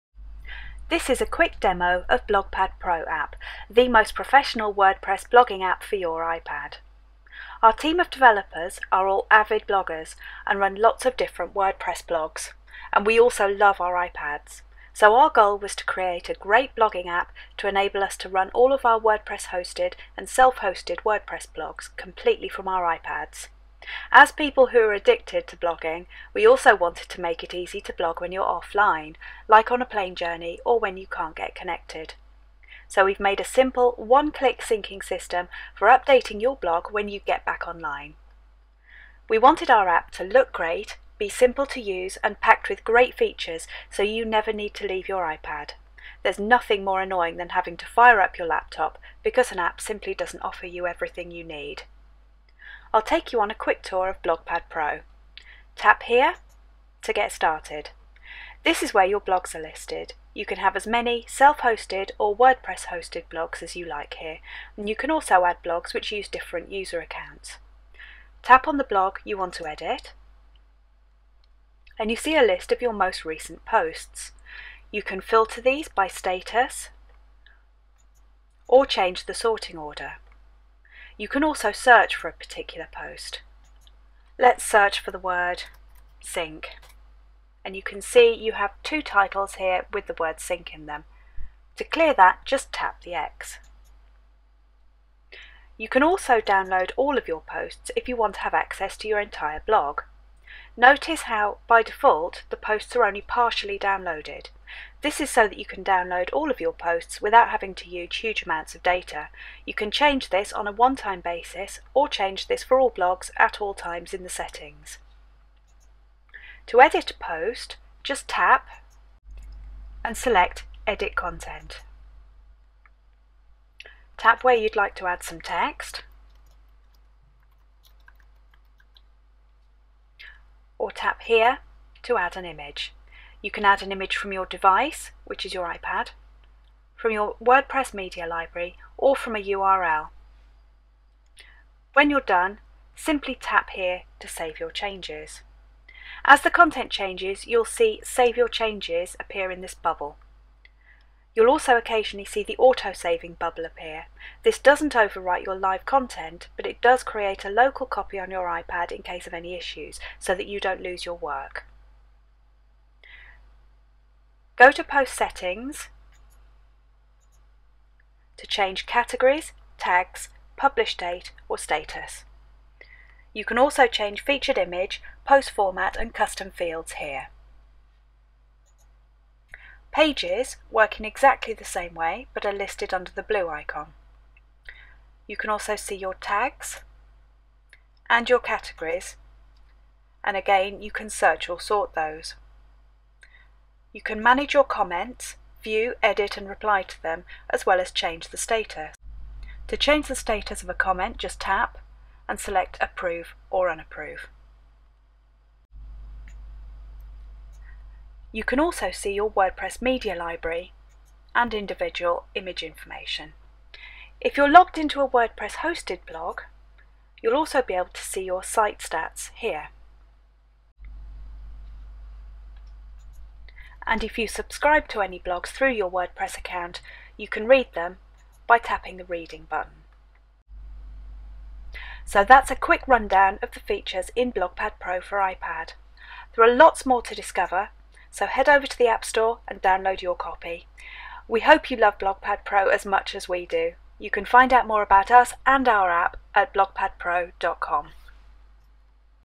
BlogPad Pro app for iPad
Available today, the app BlogPad Pro is a beautiful and intuitive blogging app which gives you everything you need to run your WordPress blogs from your iPad:
Features include: WYSIWYG editing, comment moderation, creating and editing posts and pages, WordPress.com blog stats, adding images & featured images, custom fields, sticky posts, draft posts and lots more...